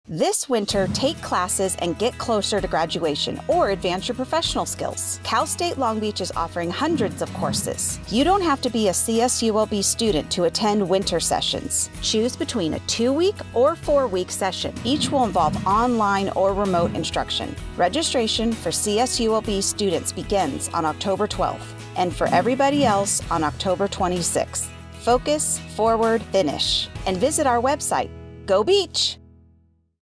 2020-21 CSULB Winter Sessions
This Winter take classes and get closer to graduation or advance your professional skills. Cal State Long Beach is offering hundreds of courses.  You don’t have to be a CSULB student to attend winter sessions.  Choose between a Two-week or Four-week session.  Each will involve online or remote instruction.
Registration for CSULB students begins on October 12th and for everybody else on October 26th. Focus, Forward, Finish. Go Beach!